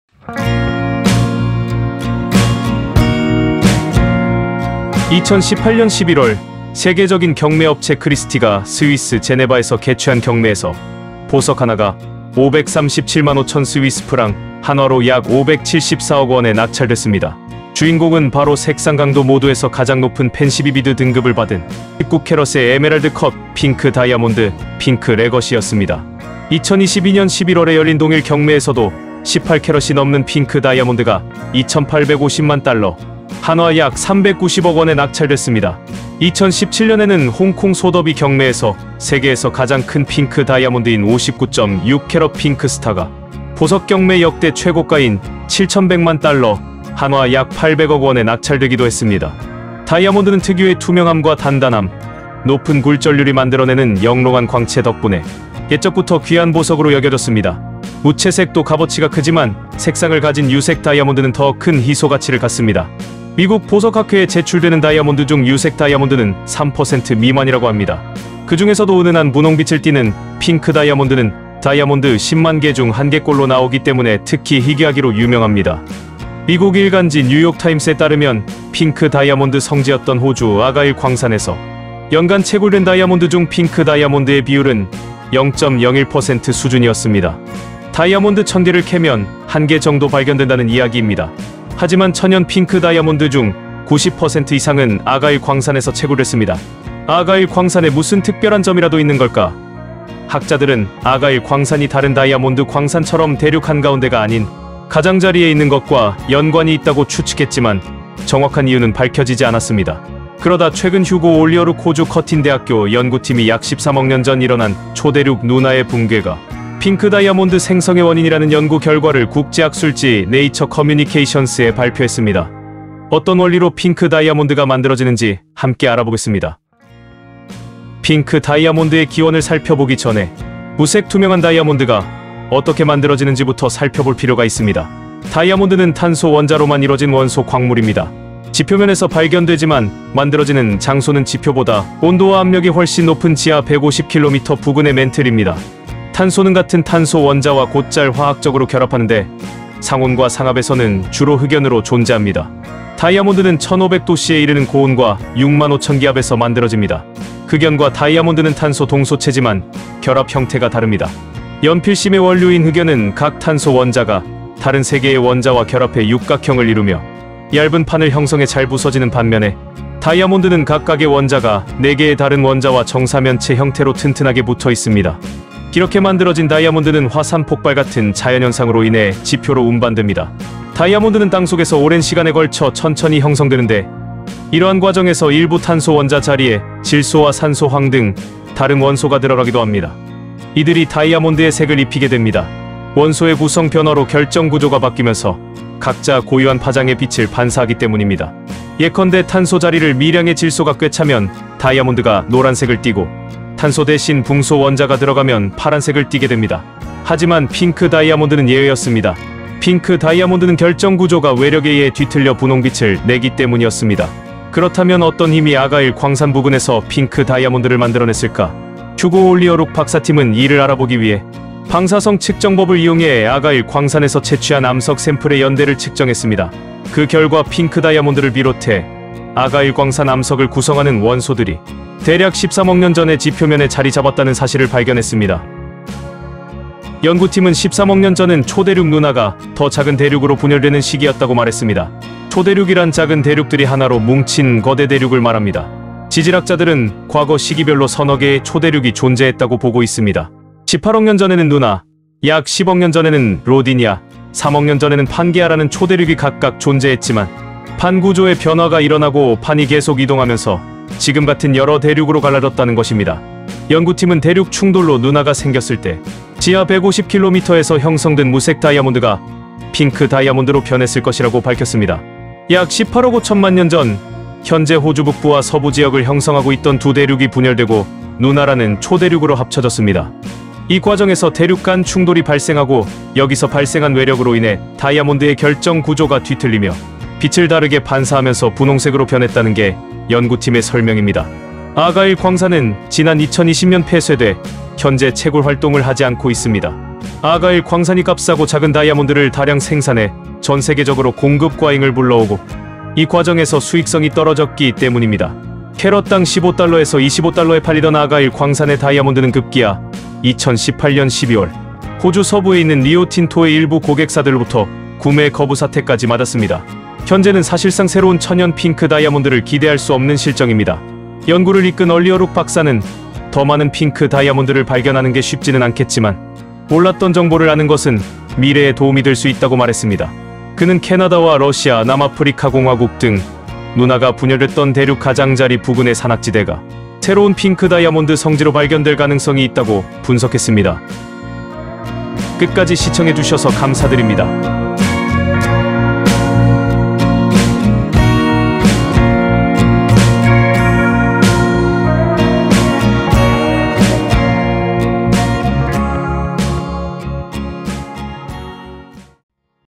Nuna가 만들어낸 핑크 다이아몬드, 비결은 초대륙 충돌
The pink diamond that Nuna created, the secret is a supercontinental collision

2018년 11월, 세계적인 경매업체 크리스티가 스위스 제네바에서 개최한 경매에서 보석 하나가 나온지 5분만에 537만5000 스위스프랑, 한화로 약 574억 원에 낙찰됐습니다. 주인공은 바로 색상 강도 모두에서 가장 높은 팬시 비비드 등급을 받은 19캐럿의 에메랄드 컷 핑크 다이아몬드 ‘핑크 레거시’였습니다.

2022년 11월에 열린 동일 경매에서도 18캐럿이 넘는 핑크 다이아몬드가 2850만 달러, 한화 약 390억 원에 낙찰됐습니다. 2017년 홍콩 소더비 경매에서는 세계에서 가장 큰 핑크 다이아몬드인 59.6캐럿 ‘핑크 스타’가 보석 경매 역대 최고가인 7100만 달러, 한화 약 800억 원에 낙찰되기도 했습니다.